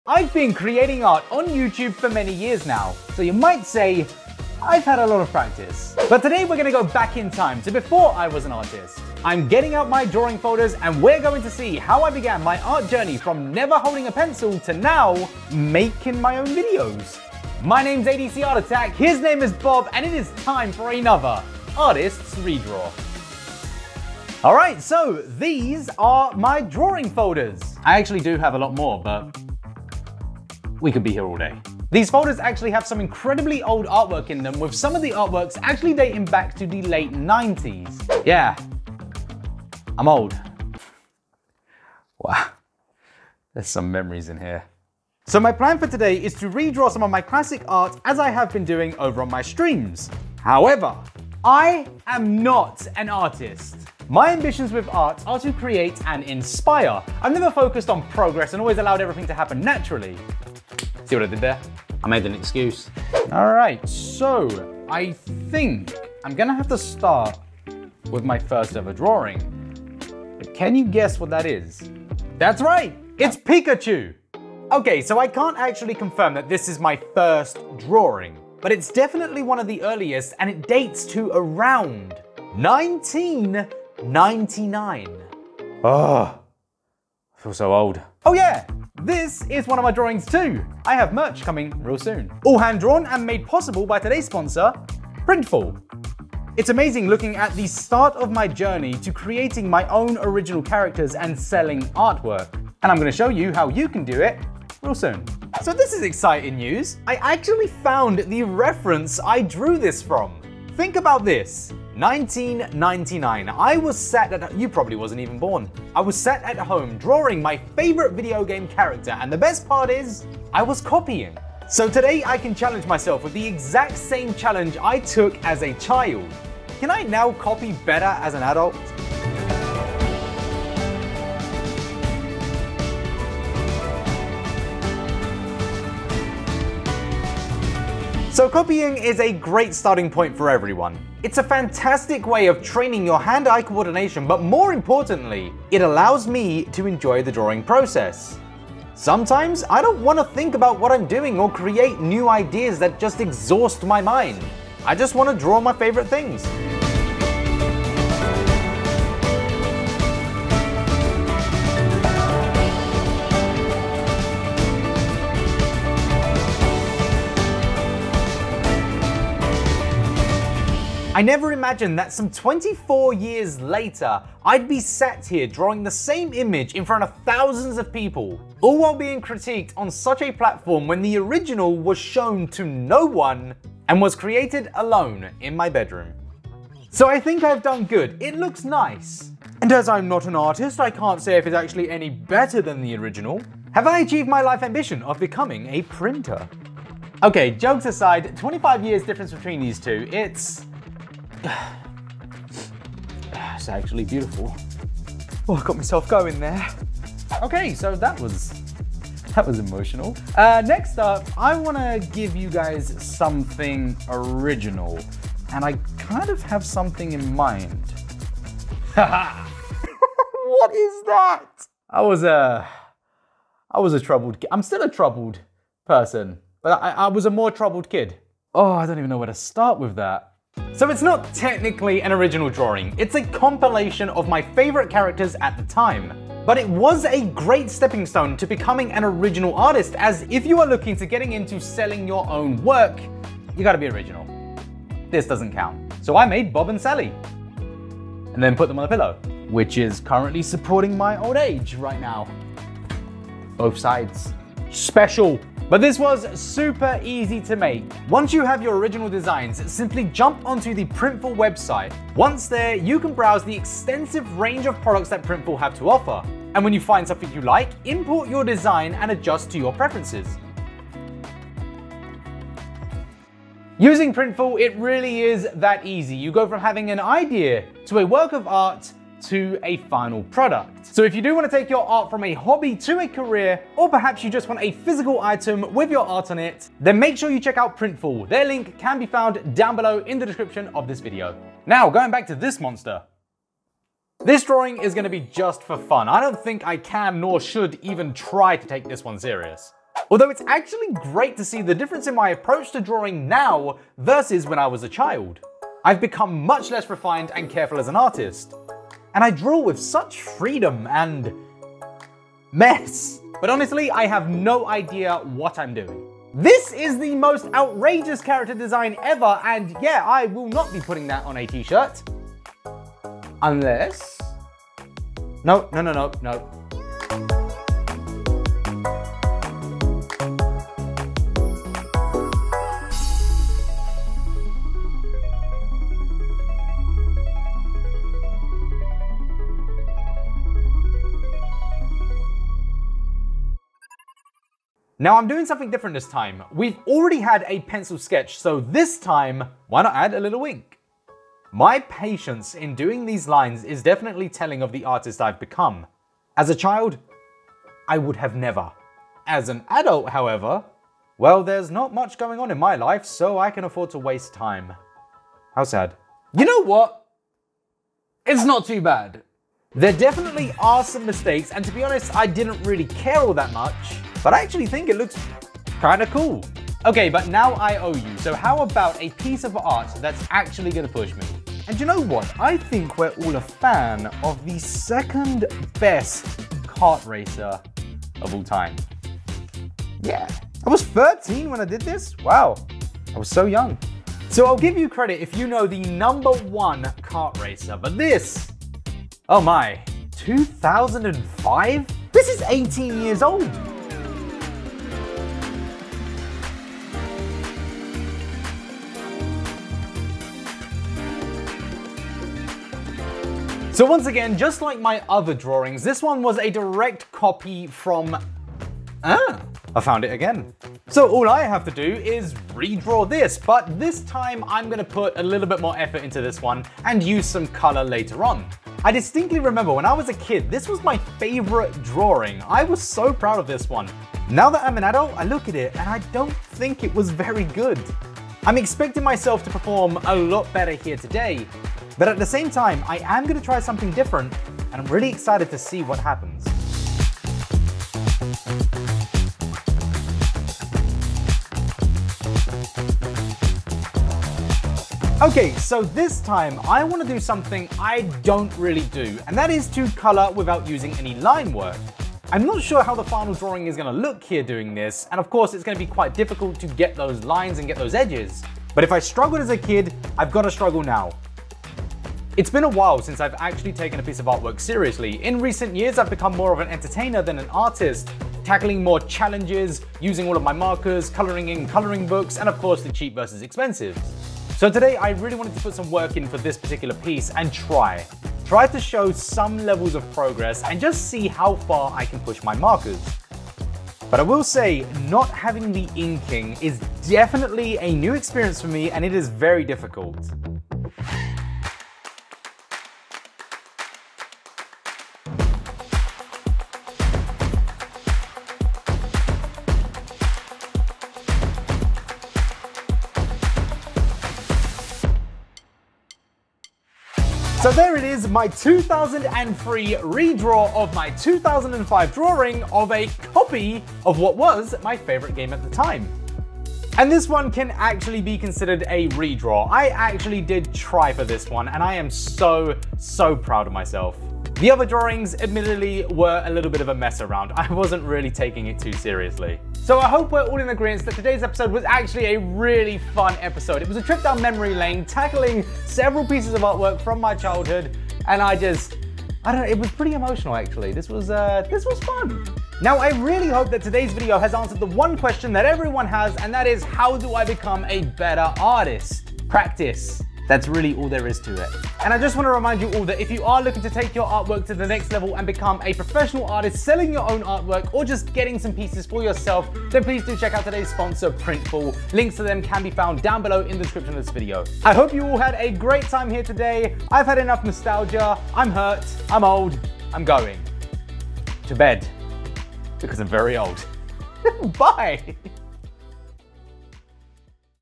Professional Artist VS Kids Art Challenge - Over 20 YEARS Difference !!
A big thank you to PRINTFUL for Sponsoring today's video with their incredible services !!
If you want to turn your Hobby into a Profession PRINTFUL Can help you

This was truly an emotional journey today... Revisiting art I thought had been lost to time. While I do not consider myself an artist anymore, having lived a fulfilling life as one for many years. I hope that my videos and content reach you, and inspire you on your journey!

Which was YOUR favorite drawing today?

Sonic The Hedgehog, Star-wars X Dragonball Z or Mario Kart Racer...

If you enjoyed today's video I will certainly provide you with another real soon.
Keep your ideas flowing and let me know of anything you would like to see me try!

All music featured as part of a Paid License via Artlist